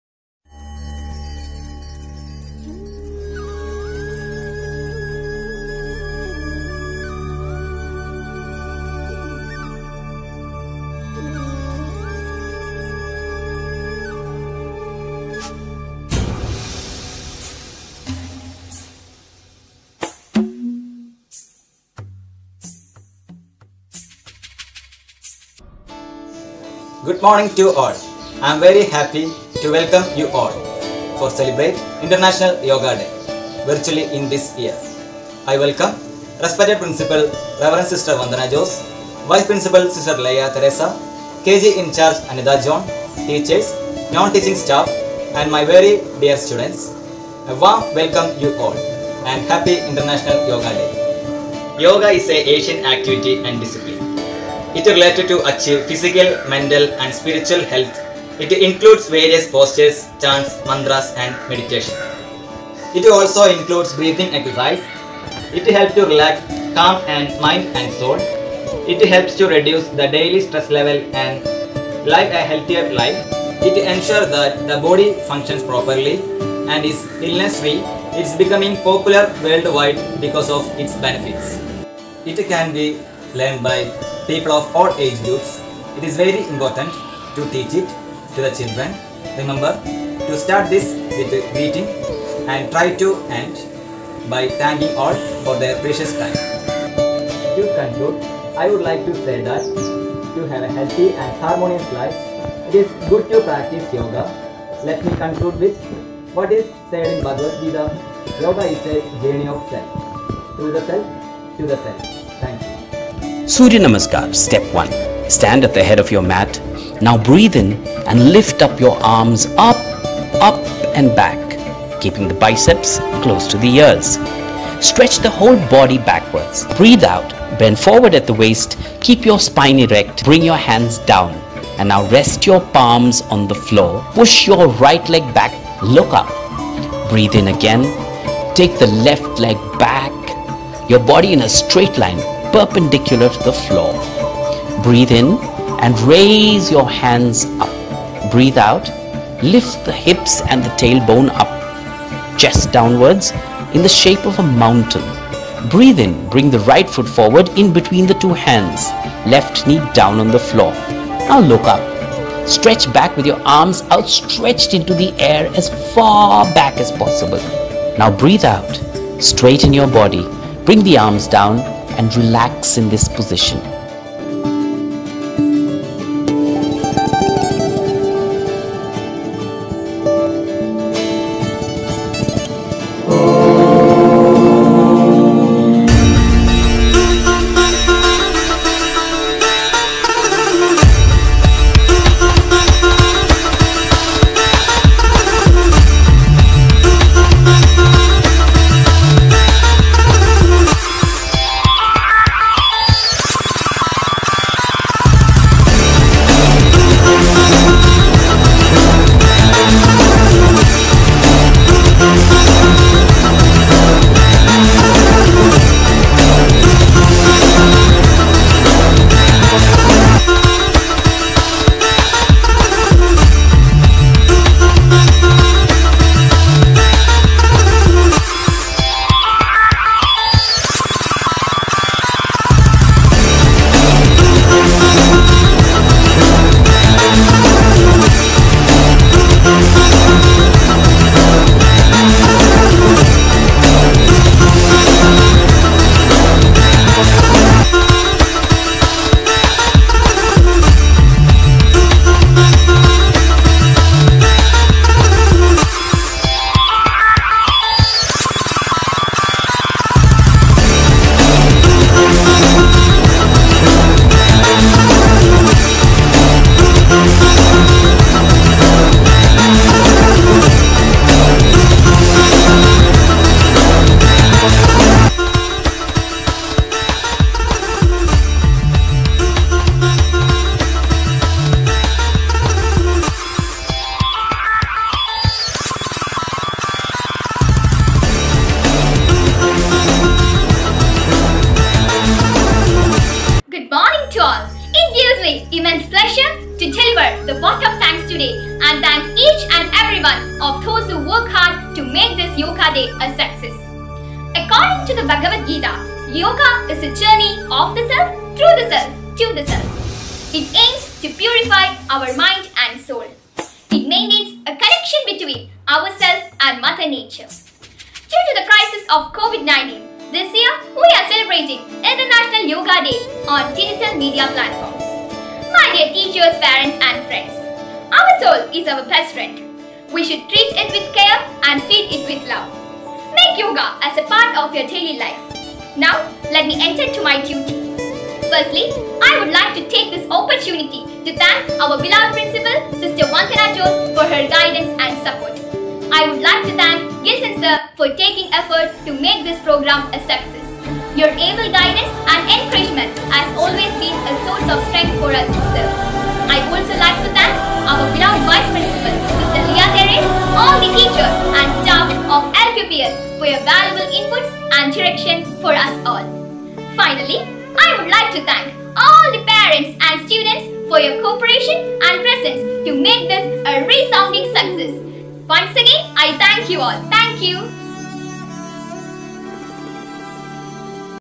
Happy International Yoga Day* 2021
Littile Queen public school is all set to bring to you the virtual events associated with the international day of yoga on June 21.

Yoga Day is celebrated across the globe to highlight the importance of Yoga and how it has been beneficial in keeping the body and mind in sound health. Considered as a holistic approach and targeting the different systems of the body and the mind, Yoga renews the mind with confidence.
  *Happy International Yoga Day*